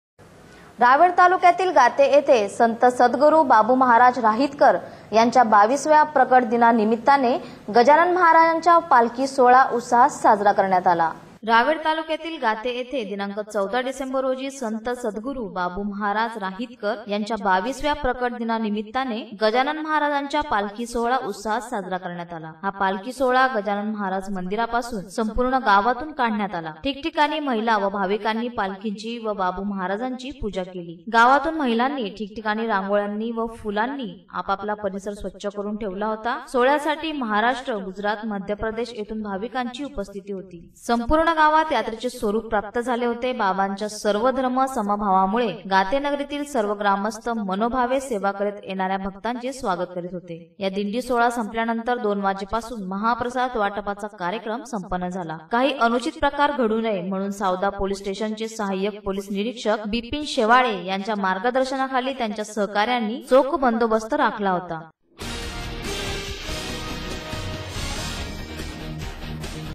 RAWER GATE SANTH SADGURU PALKI 15 12 15
रावेर येथील गाते गावात संत सदगुरू महाराज यांची पालकी सोळाह उसवात साजरा करण्यात आला.